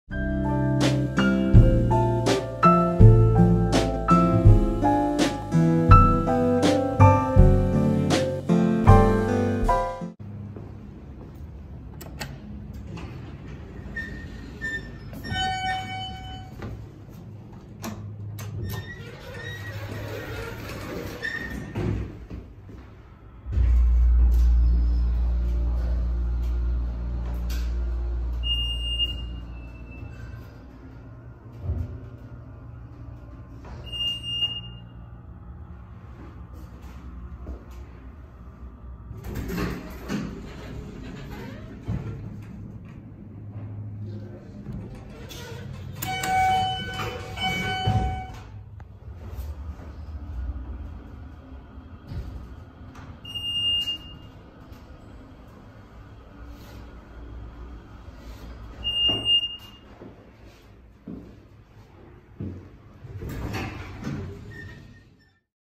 Scenic Kencor Hydraulic Elevators - Valley Forge Office Center - Wayne, PA
Filmed 5/3/21

From the information I can find online, my guess is that the atrium was built as an add-on in the mid 2000s to the two existing buildings from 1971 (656 and 676), and that these elevators were installed when the atrium/connection between the two buildings was built.